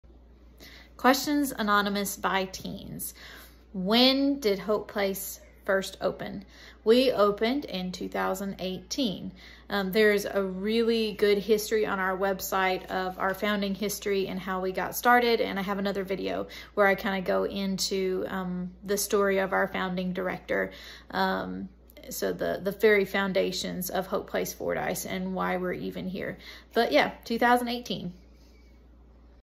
When did HopePlace Open?
This video answers a question by a Highschool teen:

“How Long have business been open?”